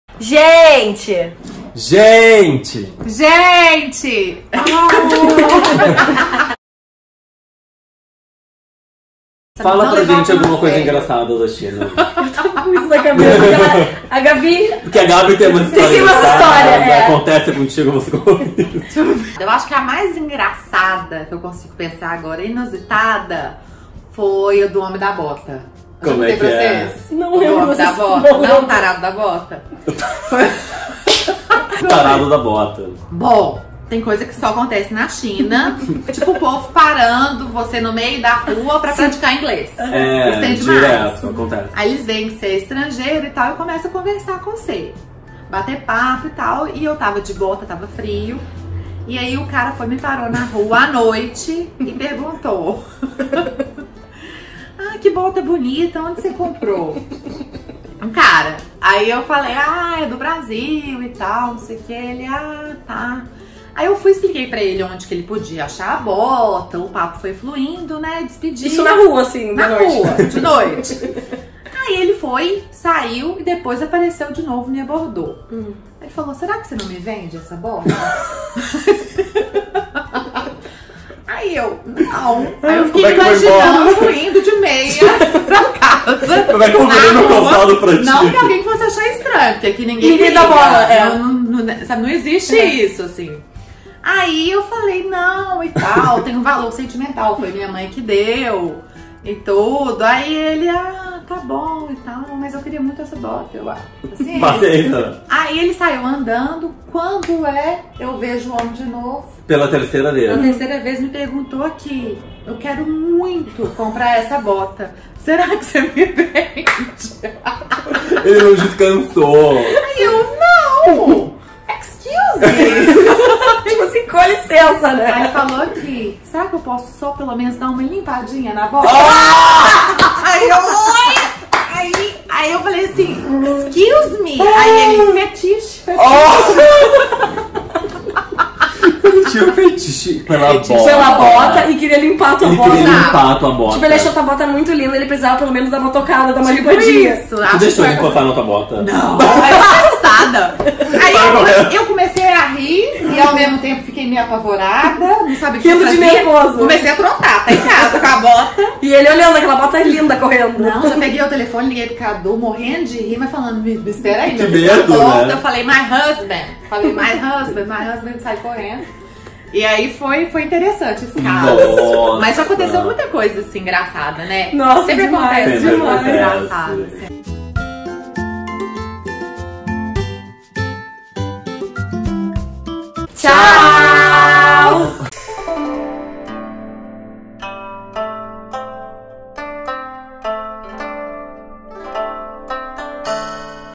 O LOUCO DA BOTA (Histórias na China)
Gente! Venham se divertir com essa história que a Gabi nos contou.